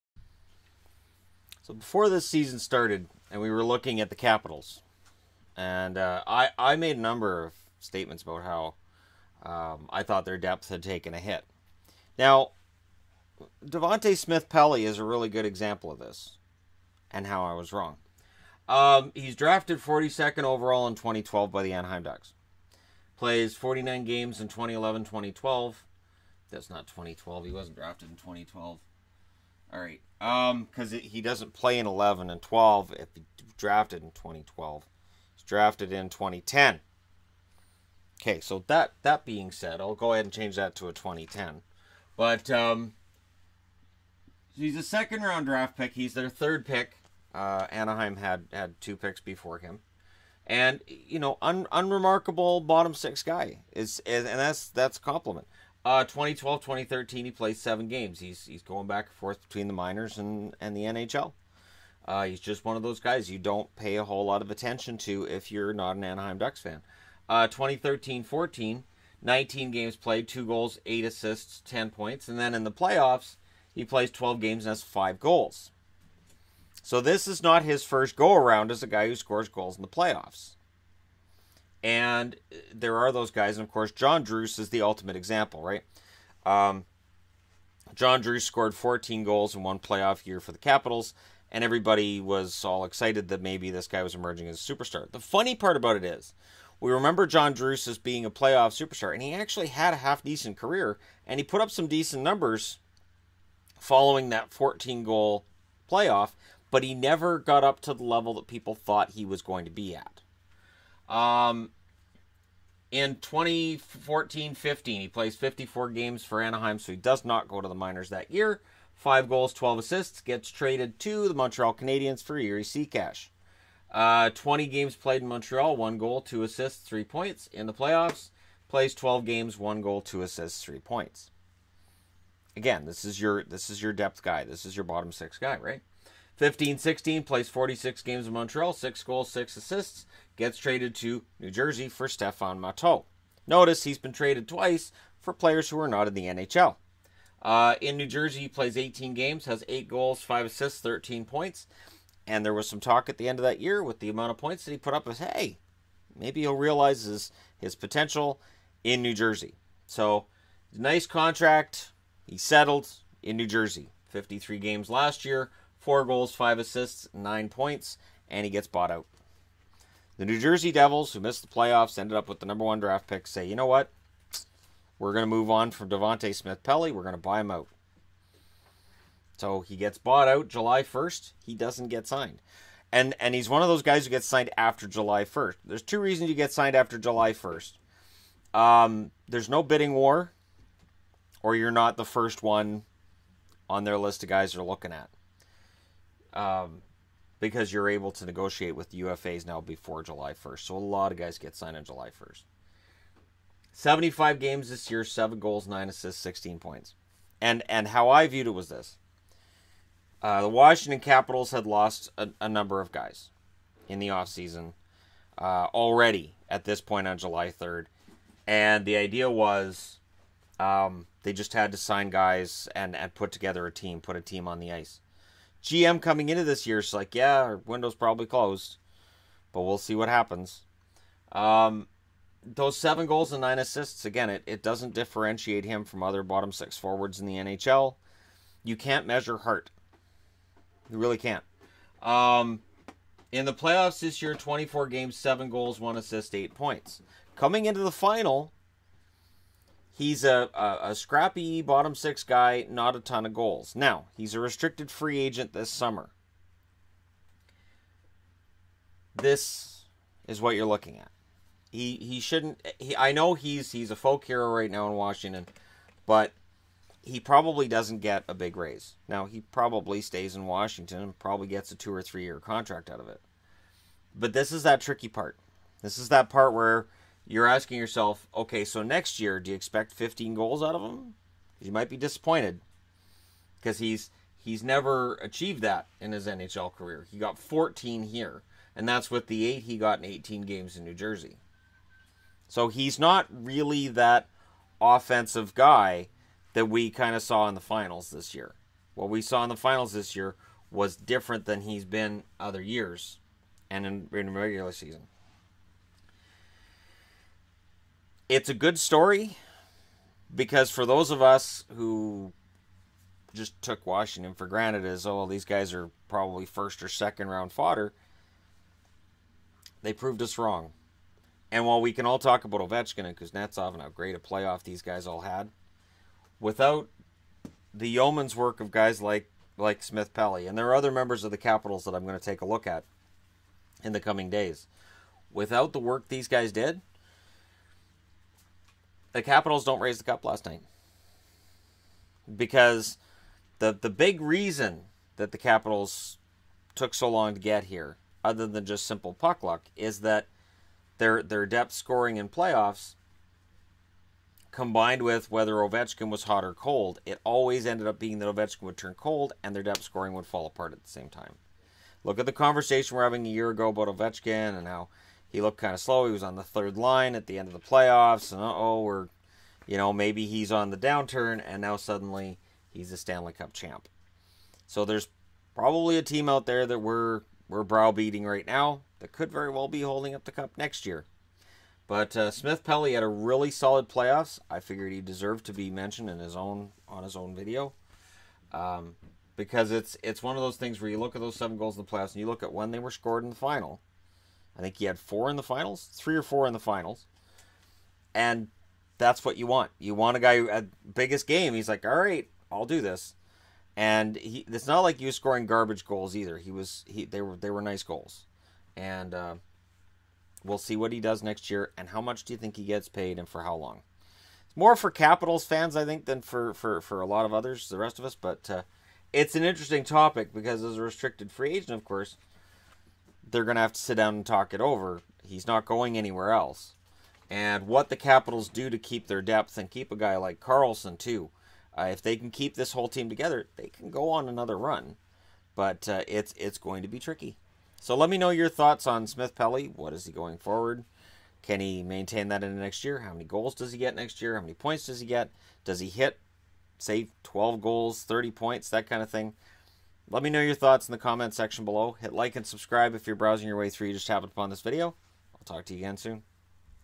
Devante Smith-Pelly's Rise to the Cup
Last summer he was bought out. This summer he is a champion.

Abbotsford, BC
V2S 8P1

In the USA:
Shannon Skanes
United States